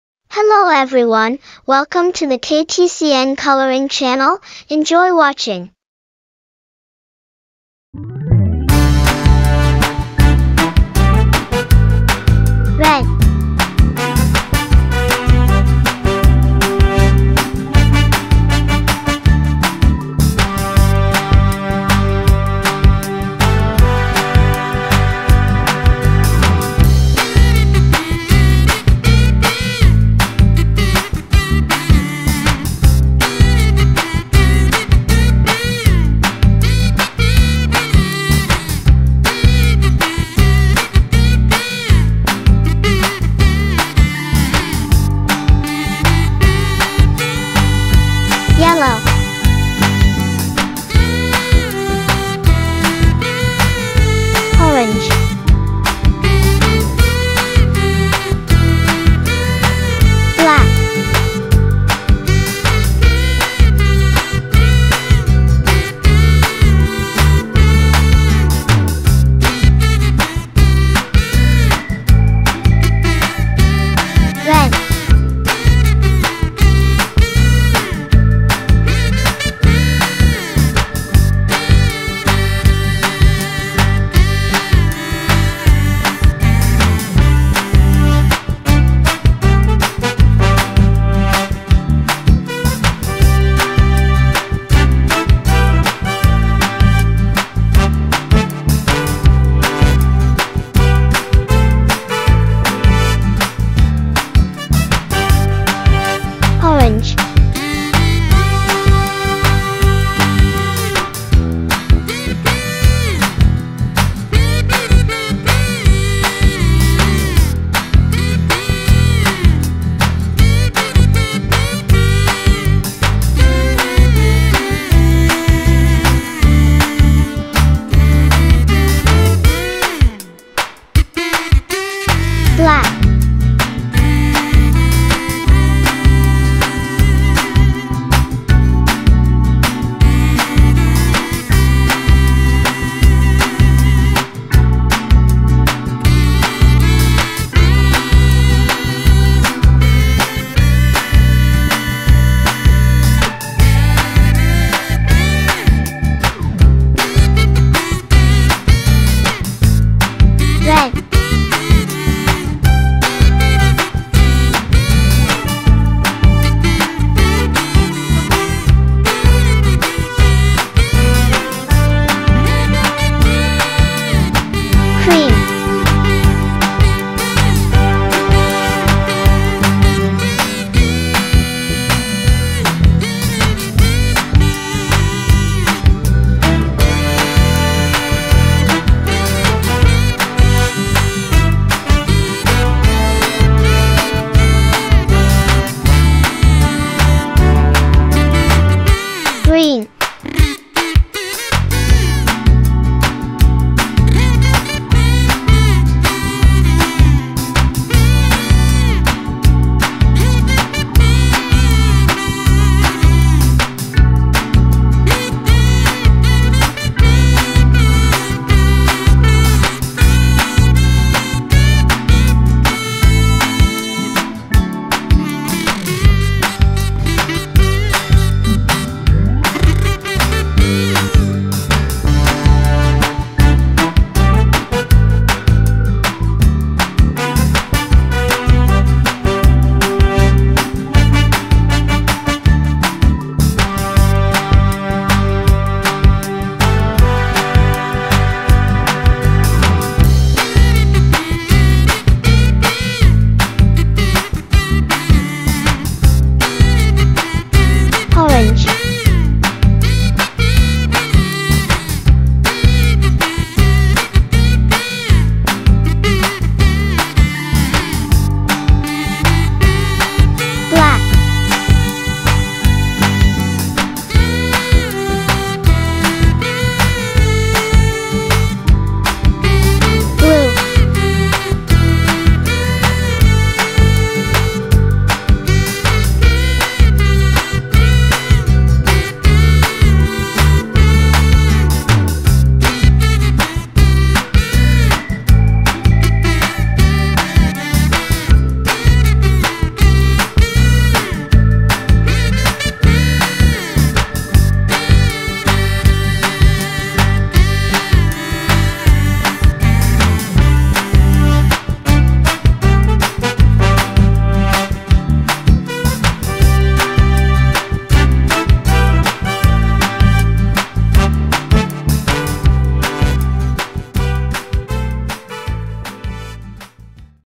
learn to color boboiboy
hello friends, today we will color a picture of Boboiboy and his friend named Adudu, I hope all of you will like this video, thank you.
learn to color boboiboy and adudu using colored pencils, coloring cartoons, drawing boboiboy